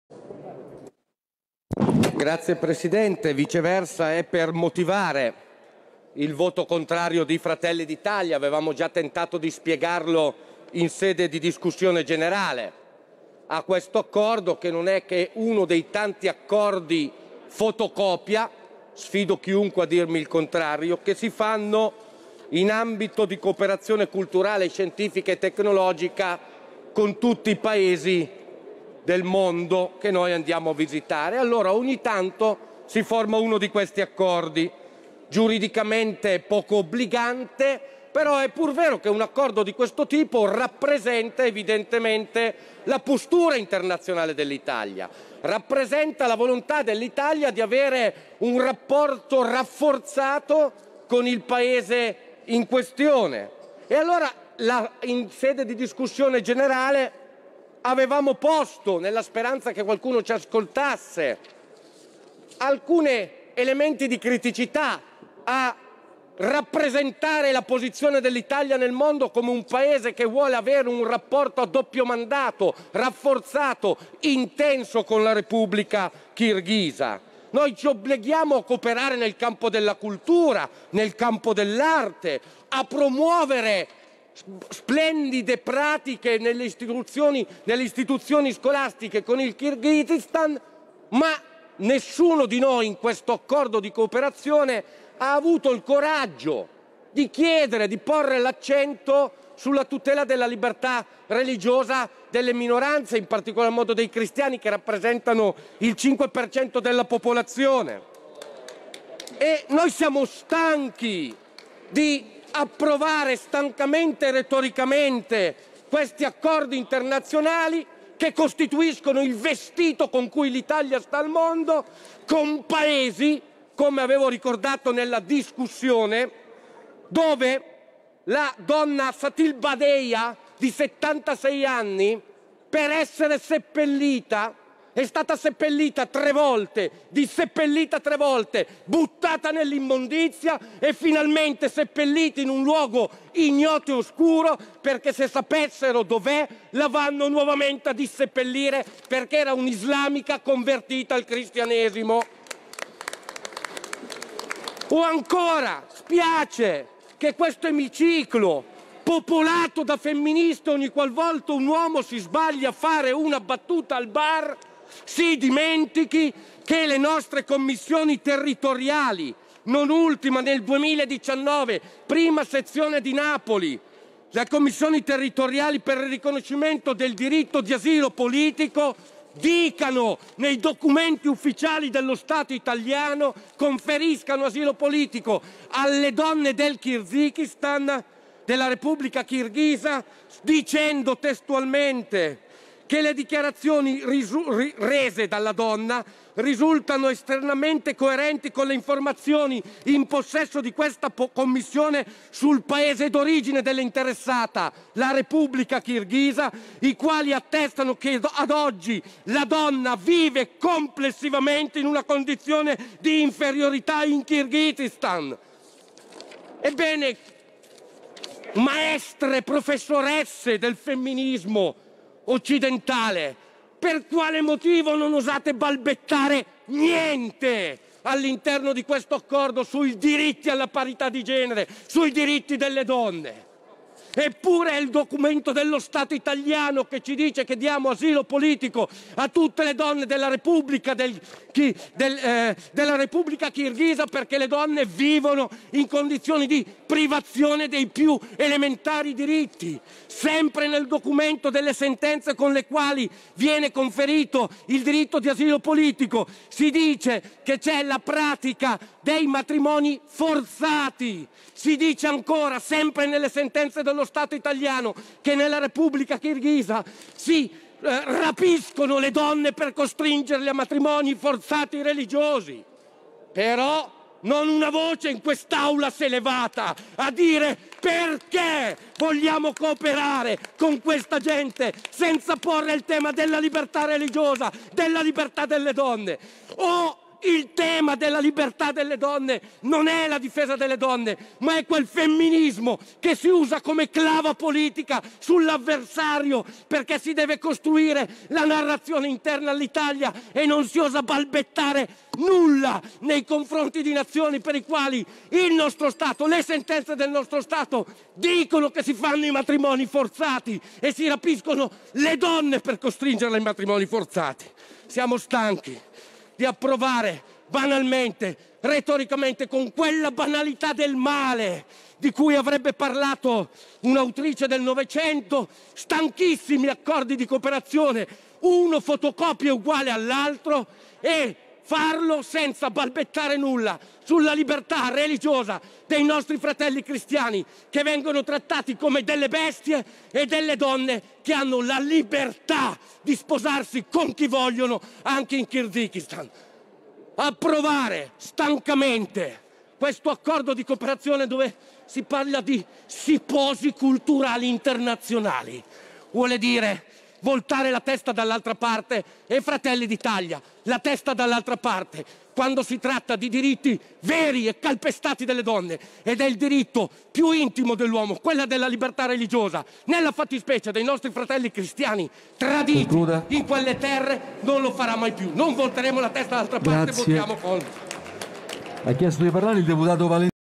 La dichiarazione di voto dell'On. Delmastro sull'accordo tra Italia e Repubblica Kirghisa
NON SI PUO’ COLLABORARE CON CHI NON RISPETTA LE MINORANZE RELIGIOSE
Questo accordo non è altro che uno dei tanti accordi fotocopia in ambito di cooperazione culturale. Noi ci obblighiamo a promuovere splendide pratiche con il Kirghizistan ma nessuno ha avuto il coraggio di porre l’accento sulla tutela delle minoranze religiose, come quella cristiana. Siamo stanchi di approvare con la banalità del male accordi di collaborazione con paesi che non rispettano i nostri fratelli cristiani e le donne,  i cui diritti vengono sistematicamente calpestati
La dichiarazione di voto di Andrea Delmastro sulla
Ratifica ed esecuzione dell'Accordo tra il Governo della Repubblica Italiana ed il Governo della Repubblica Kirghisa sulla cooperazione culturale, scientifica e tecnologica, fatto a Bishkek il 14 febbraio 201